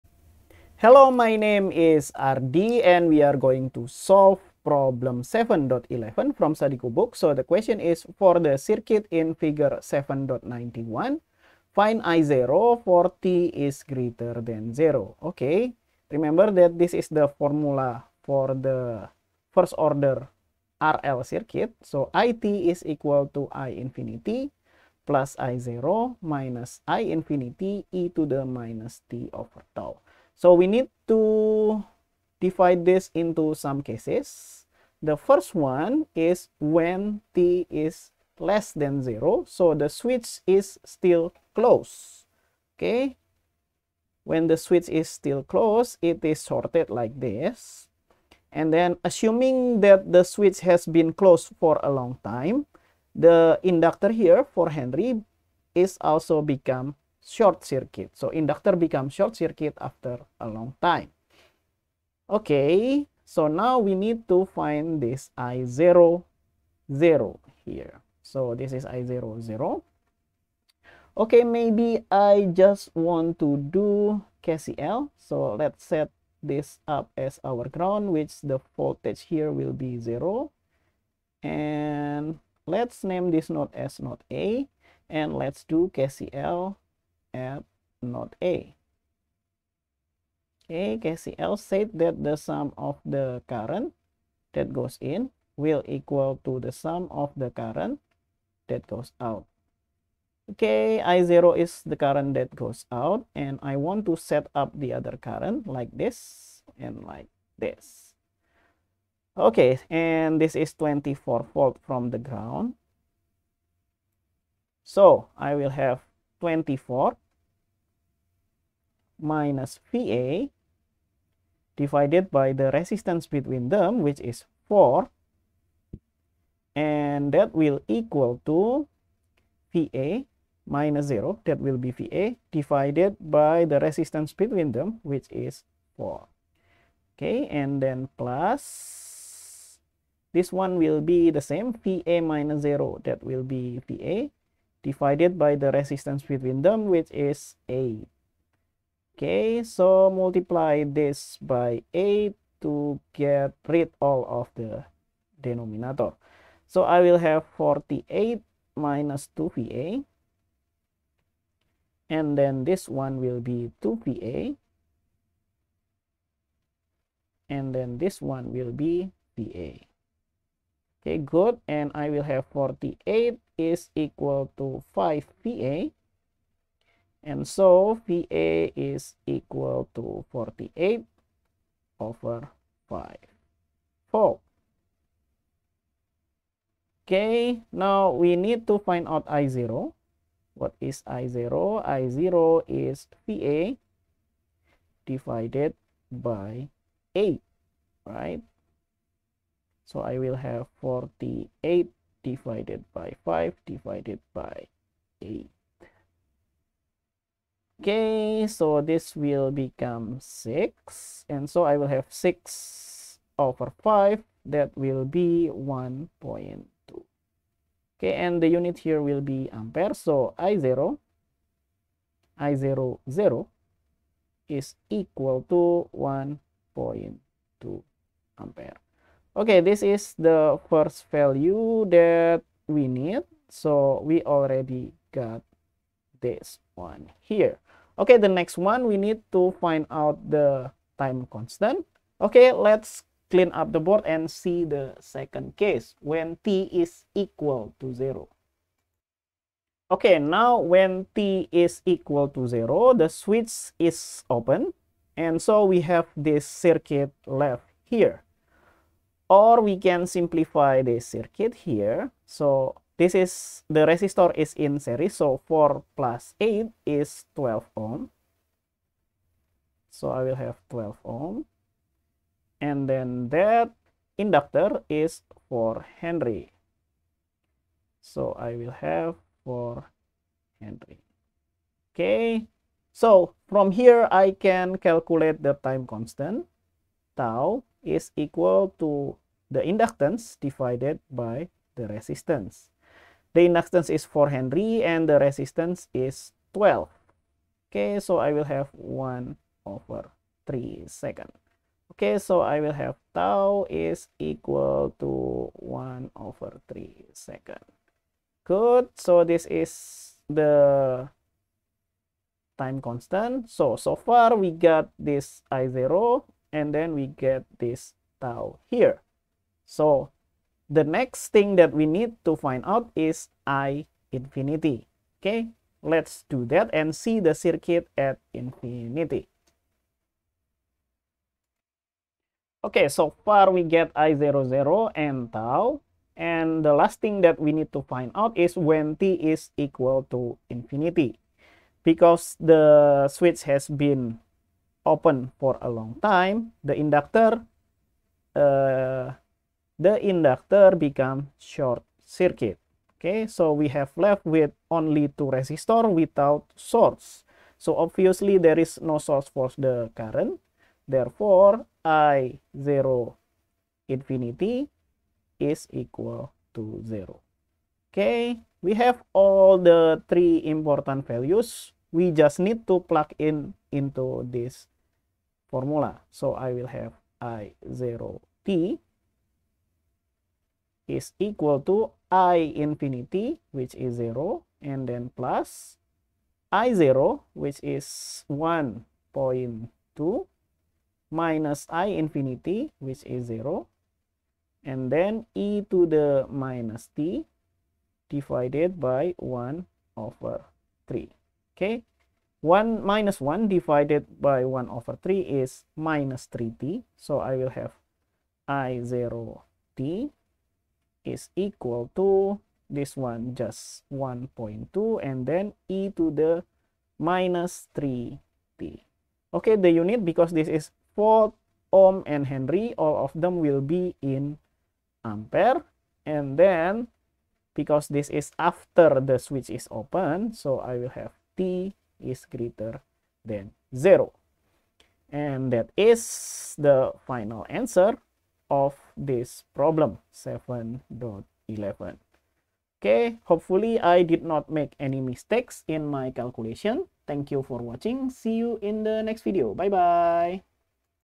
Problem 7.11 - Fundamental of Electric Circuits (Sadiku) 5th Ed
7.11 For the circuit in Fig. 7.91, find io for t ≥ 0.